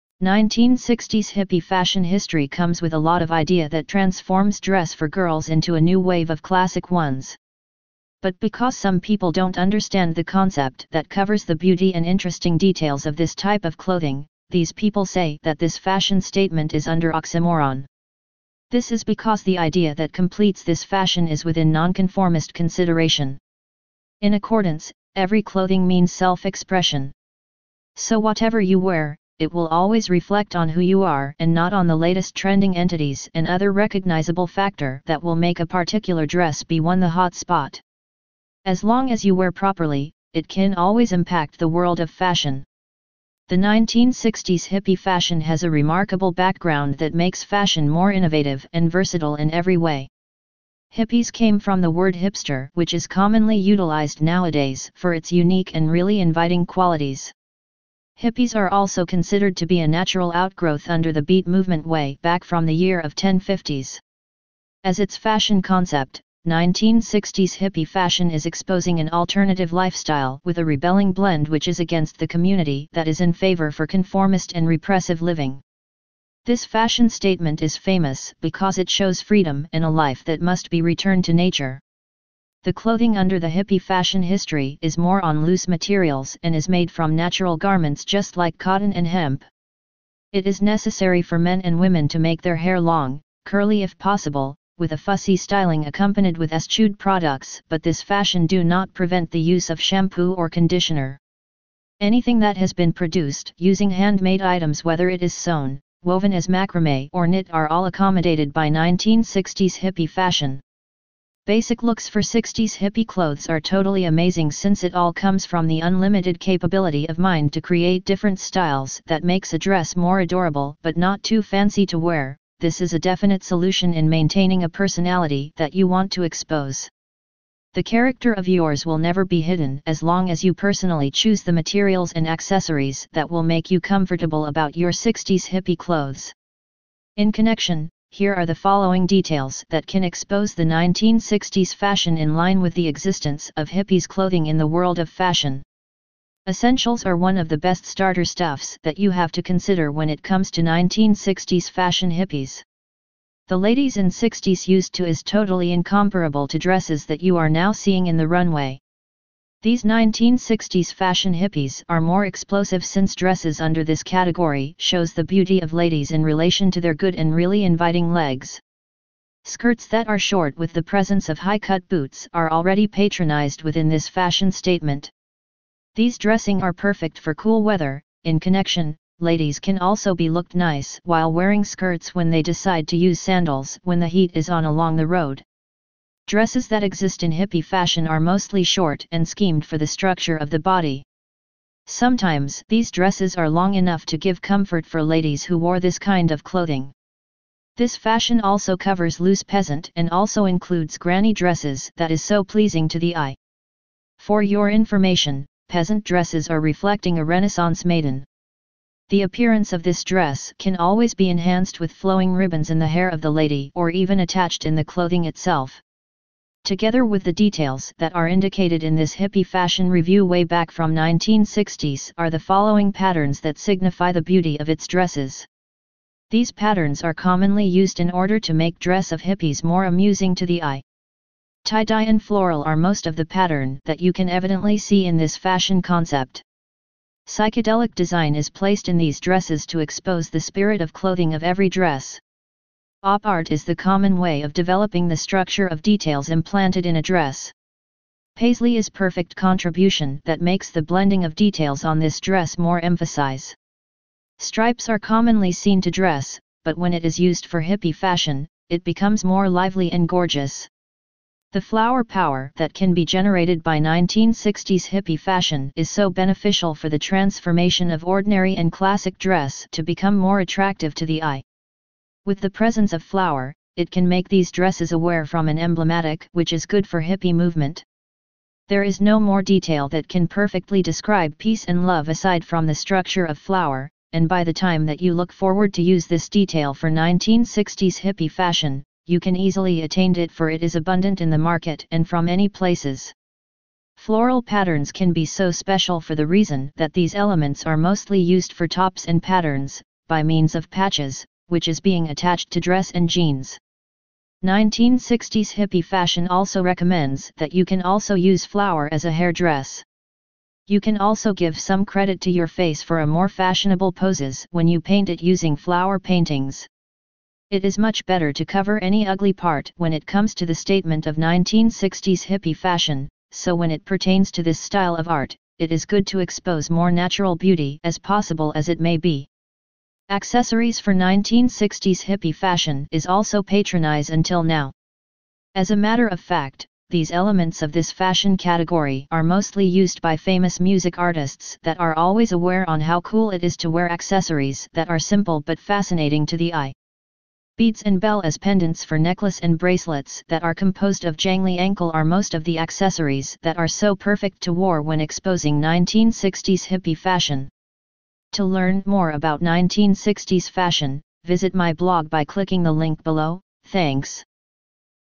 1960s Hippie Fashion History
1960s Hippie Fashion history comes with a lot of idea that transforms dress for girls into a new wave of classic ones. But because some people don't understand the concept that covers the beauty and interesting details of this type of clothing, these people say that this fashion statement is under oxymoron. This is because the idea that completes this fashion is within nonconformist consideration. In accordance, every clothing means self-expression. So whatever you wear, it will always reflect on who you are and not on the latest trending entities and other recognizable factor that will make a particular dress be one the hot spot. As long as you wear properly, it can always impact the world of fashion.

1960s hippie fashion history,
1960s hippie fashion,
hippie fashion history,
60s hippie clothes,
1960s Fashion